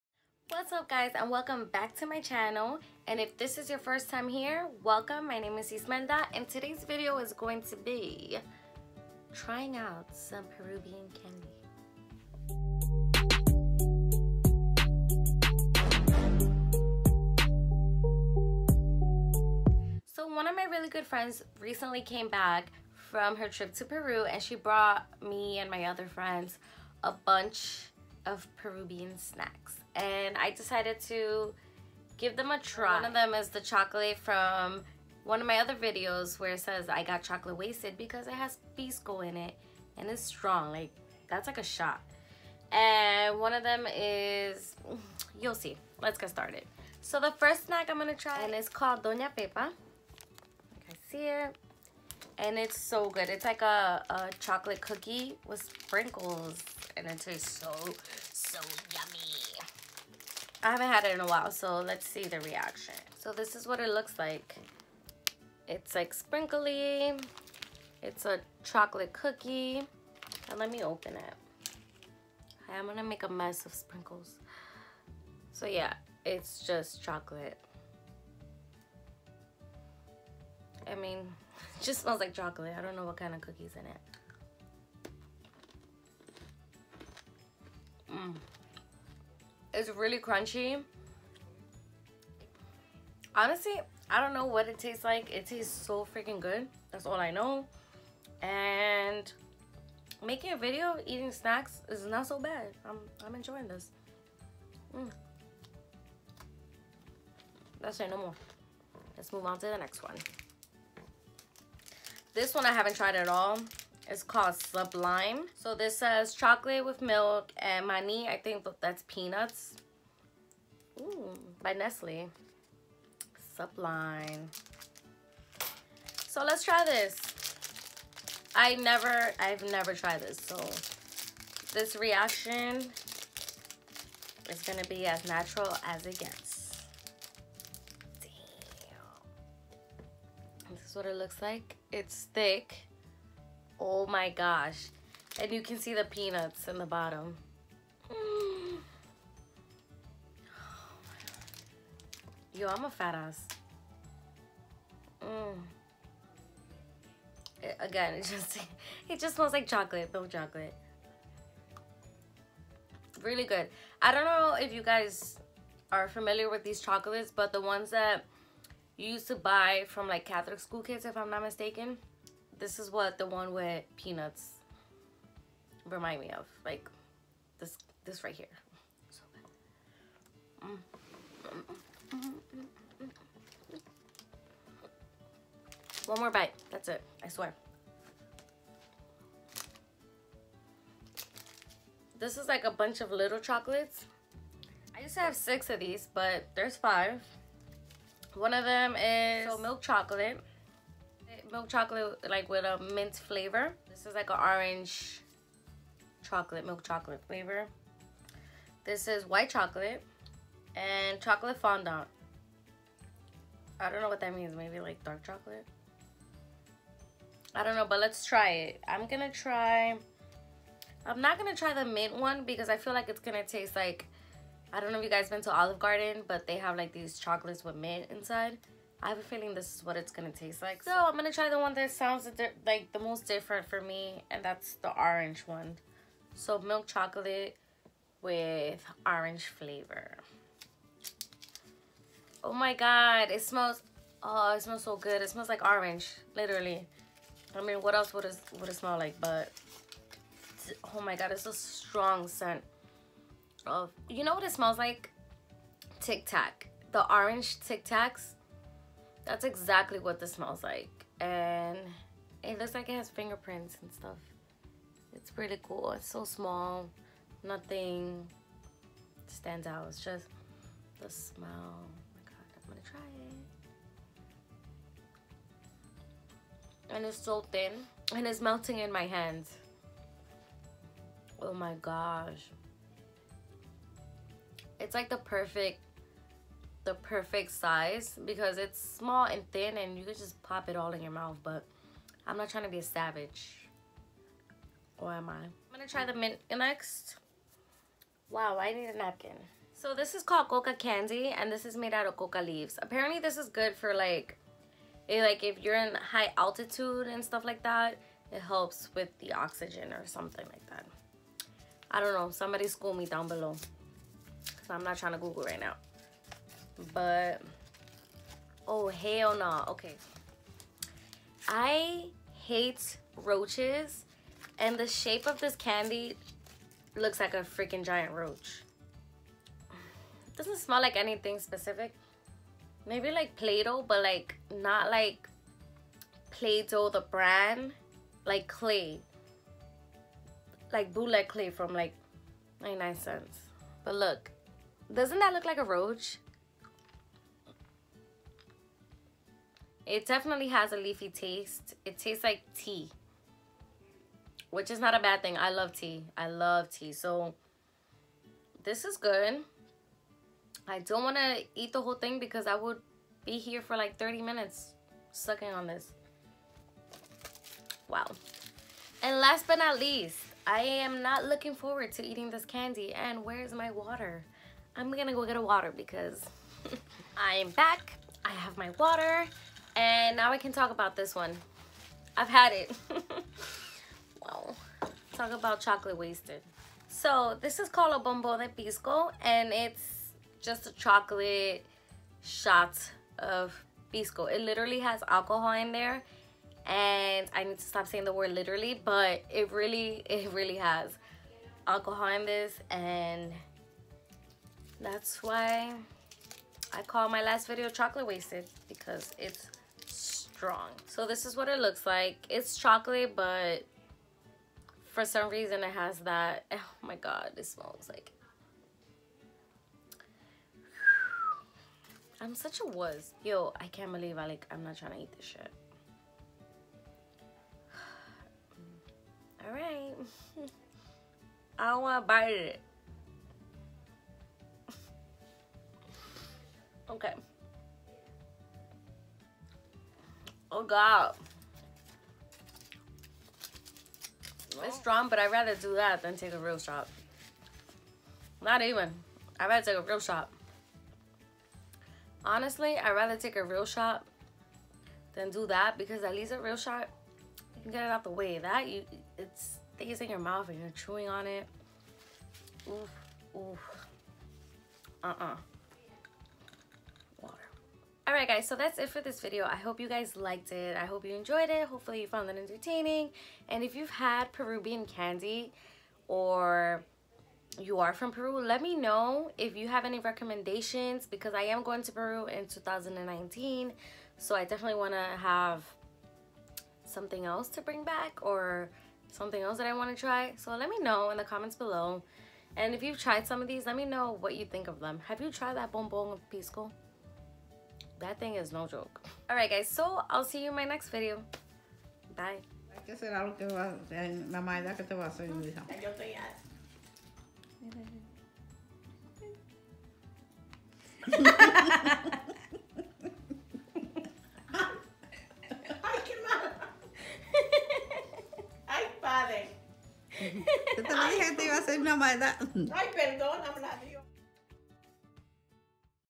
Trying Out Snacks From Peru 🇵🇪
One of my good friends just got back from her trip to Peru, she brought me some snacks to try out.
Enjoy, Xoxo